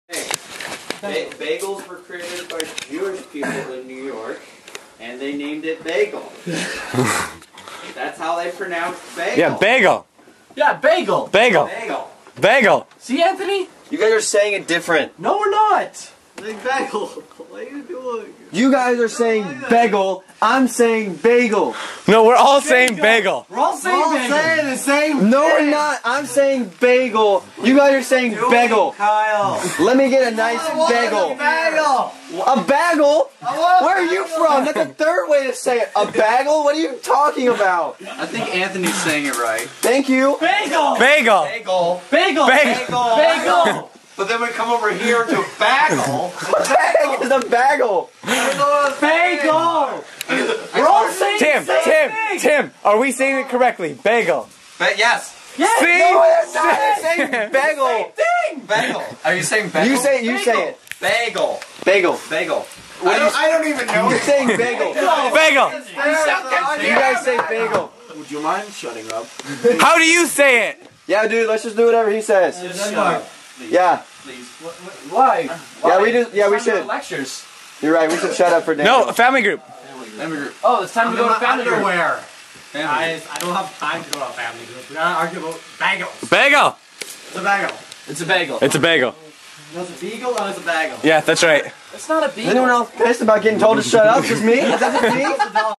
BAGEL!!!!
When there are Canadians and Americans living under one roof there will be misunderstandings when it comes to how to pronounce certain words, like bagel.